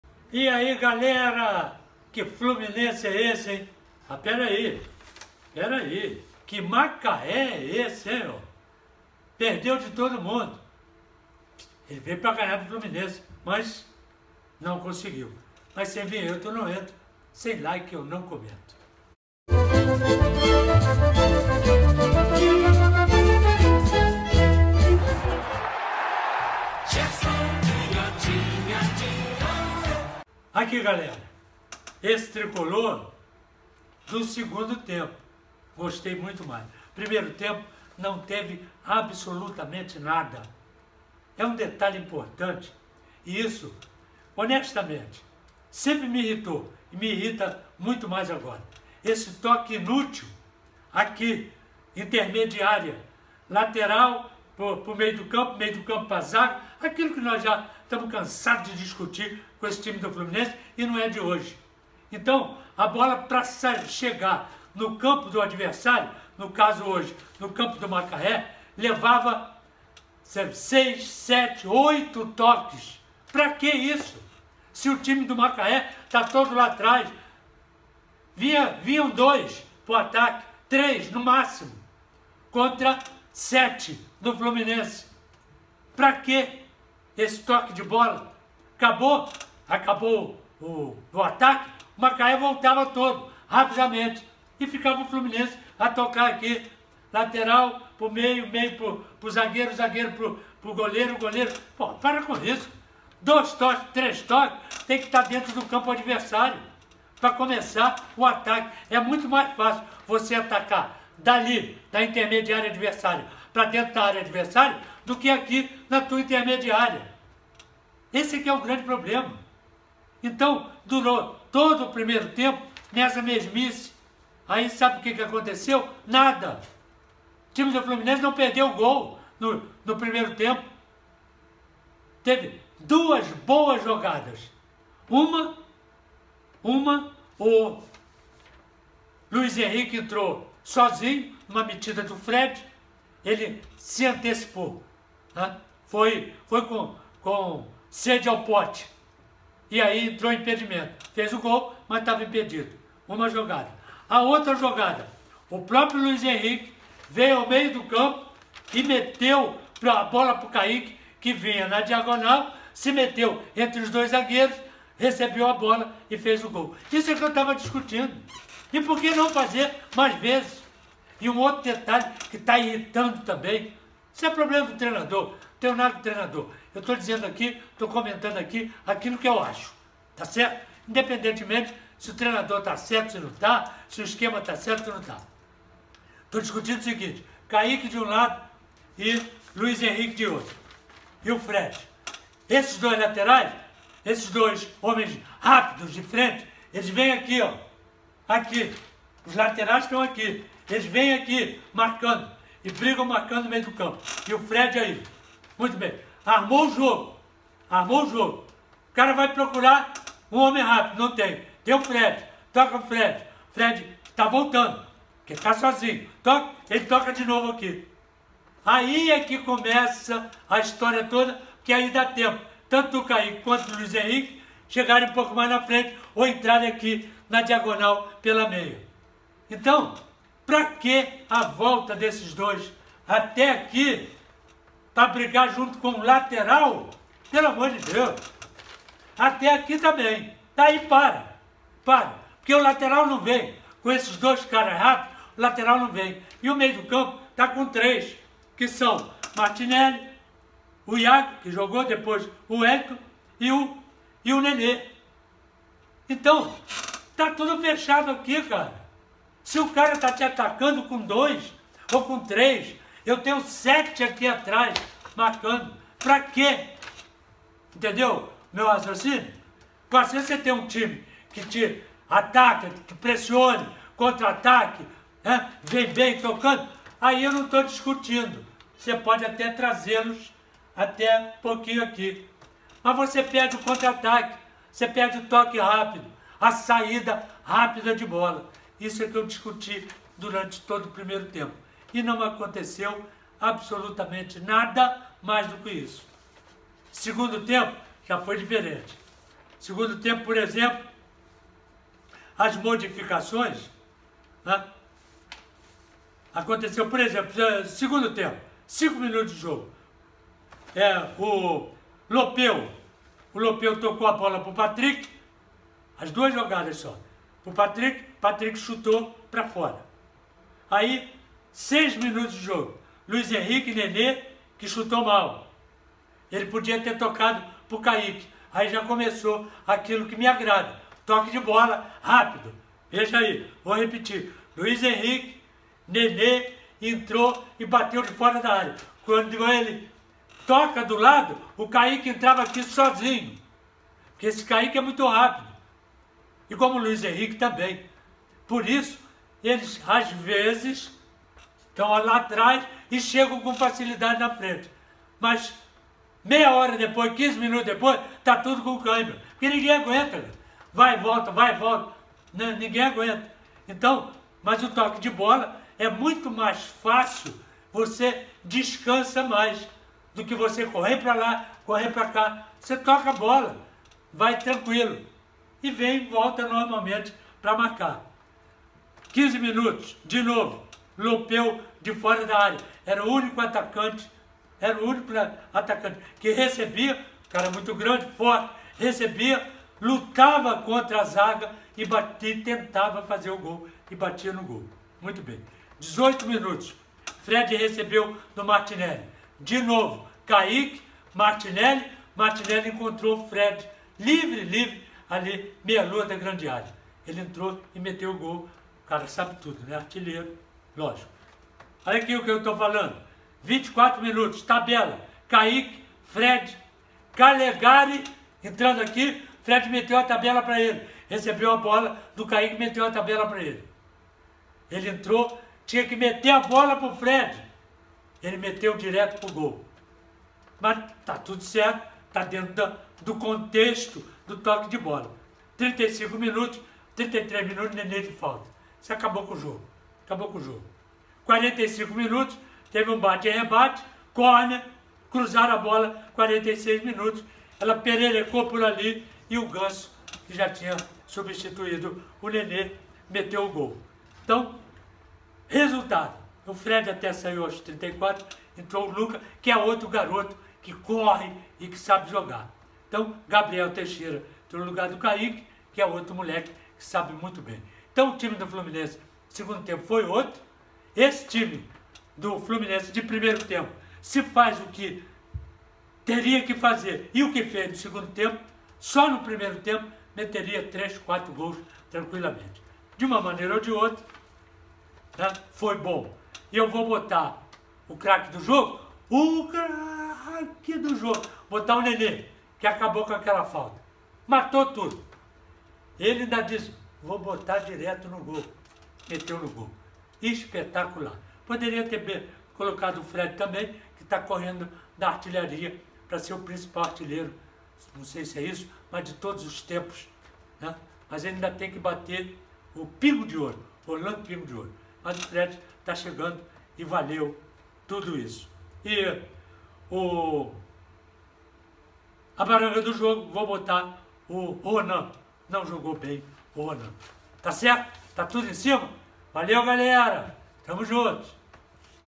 CHEGOU NO G4 - MACAÉ 0X4 FLUMINENSE - CAMPEONATO CARIOCA 2021
Pressão tricolor no comecinho do jogo, e depois esfriou. Fez duas boas jogadas, conseguiu um gol e foi só. Esperava mais. No segundo tempo, o Flu veio mais disposto, as jogadas começaram a aparecer, e os gols foram saindo naturalmente. Gostei das entradas do Wellington e do Gabriel Teixeira, que quase fez um golaço!
O craaaaaaaaque do jogo, Nenê!

Publicidade ligue: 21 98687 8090